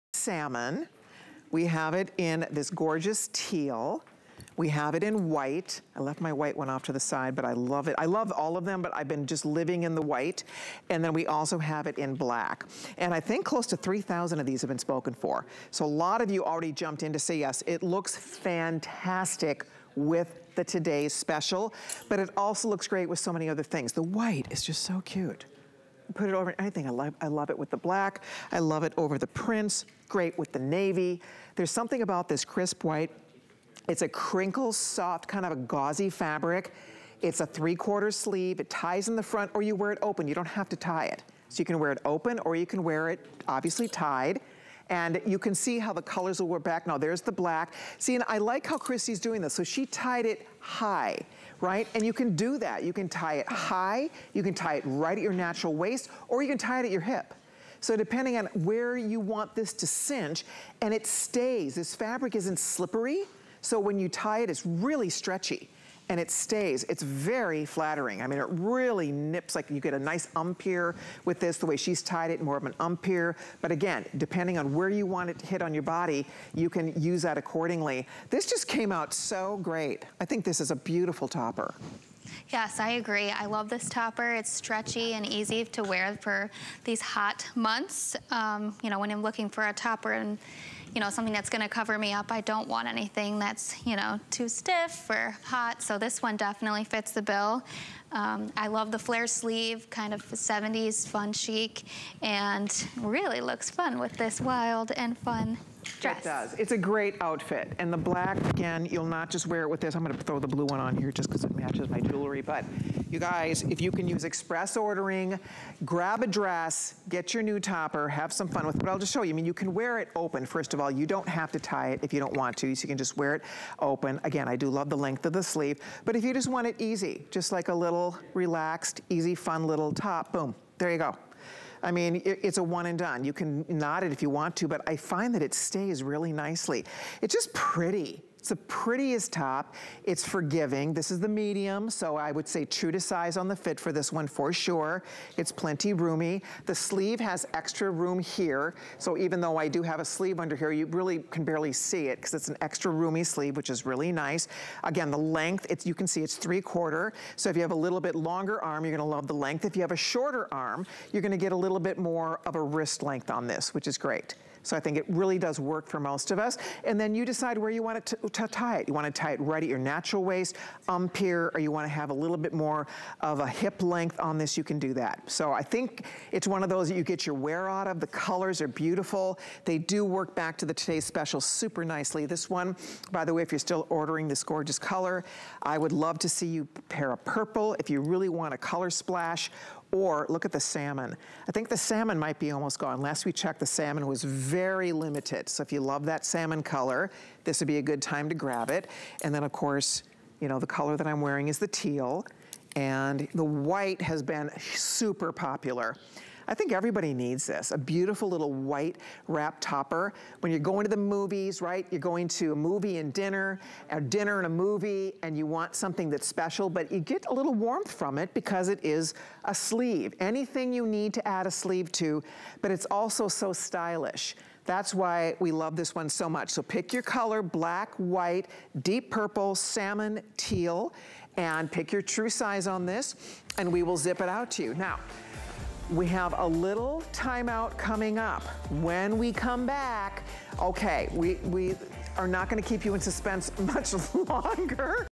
Colleen Lopez Ruffle Sleeve Topper
This soft, lightweight rufflesleeve topper is the perfect complement to your sleeveless garments, adding justright coverage and a nice, loose fit.